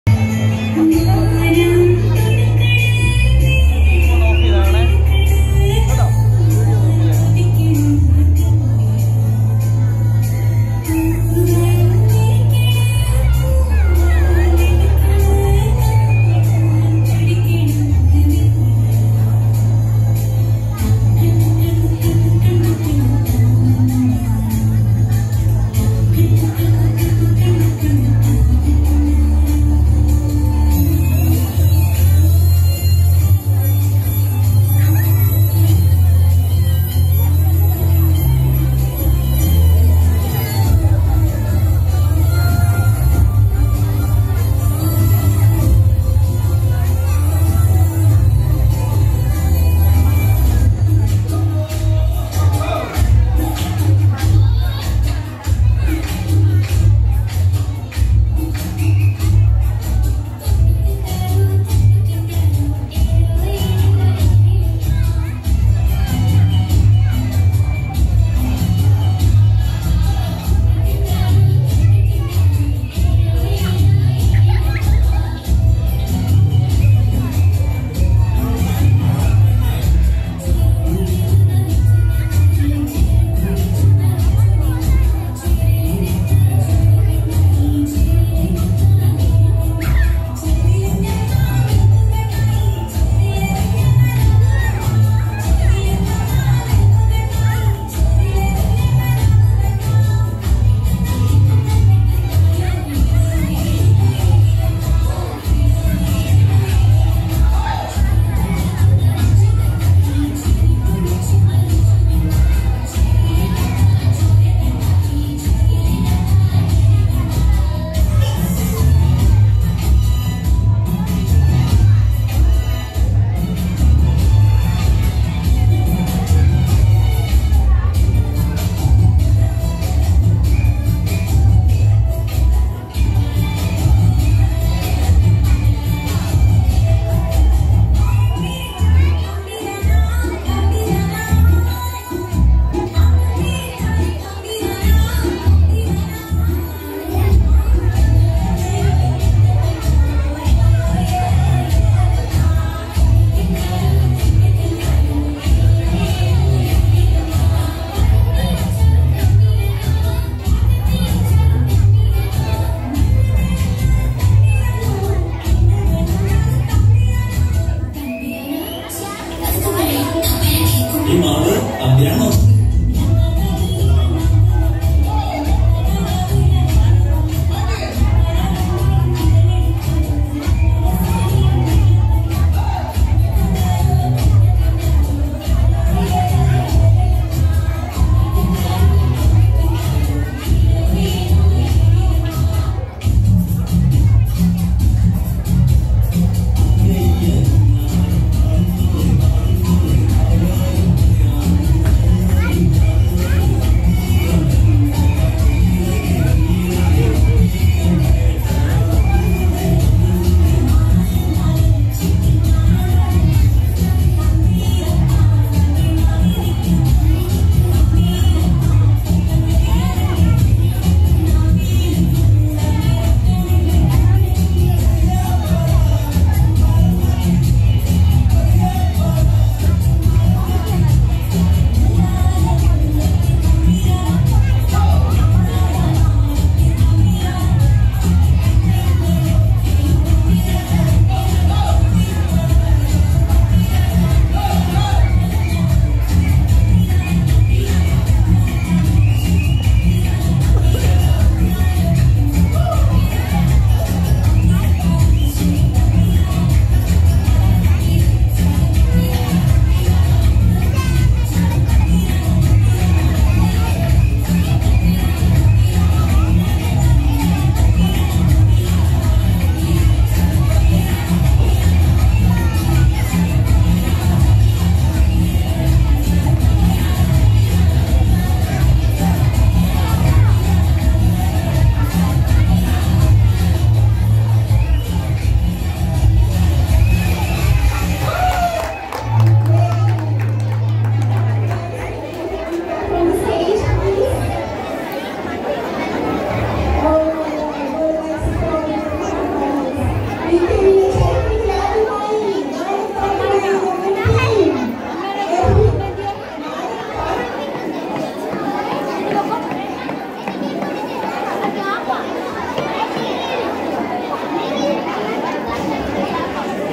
NADODINRUTHAM - ANNUALDAY SARGALAYA 2K23 Govt.L.P.S THIRDCAMP
GOVT.L.P.S THIRDCAMP CELEBRATED ANNUAL DAY-SARGALAYA 2K23 WITH A VARIETY OF CULTURAL PROGRAMMES ENSURING THE PARTICIPATION OF ALL CHILDREN